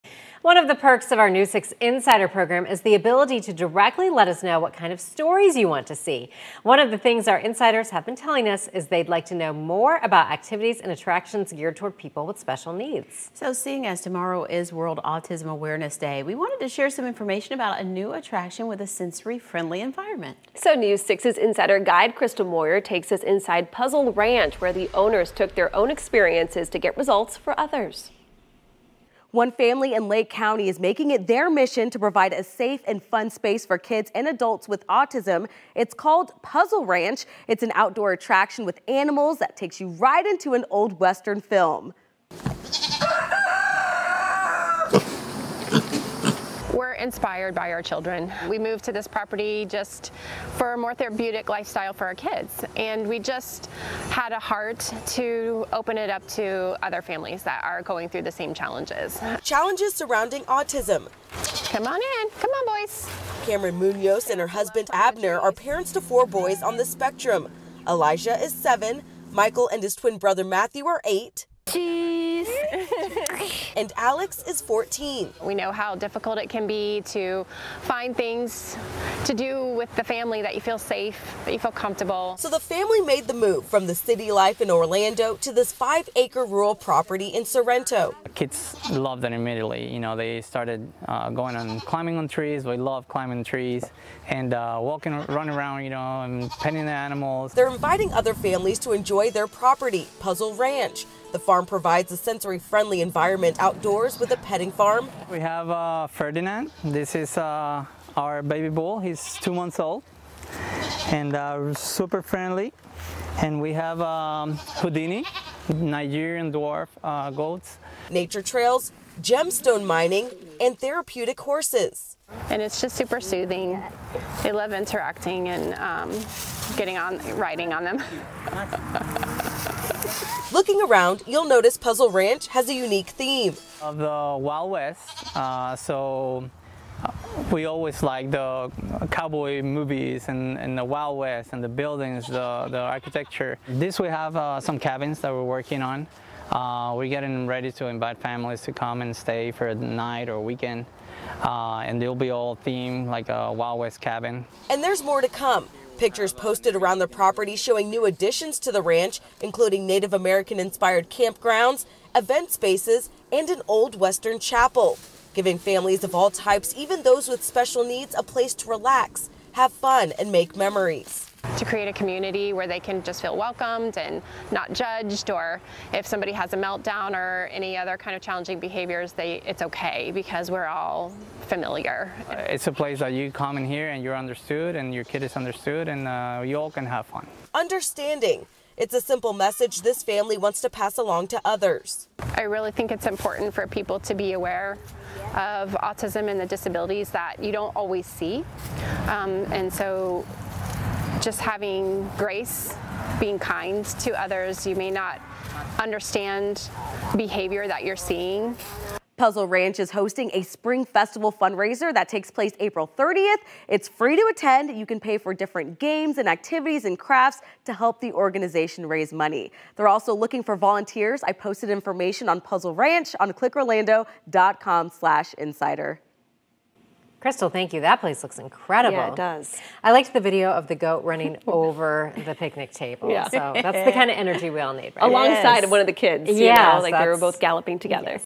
Puzzle Ranch provides visitors with sensory-friendly, outdoor destination in Lake County
A family in Lake County is making it their mission to provide a safe and fun space for kids and adults who have autism or other disabilities. The destination is called Puzzle Ranch. It’s an outdoor attraction with animals that takes you right into an old western film.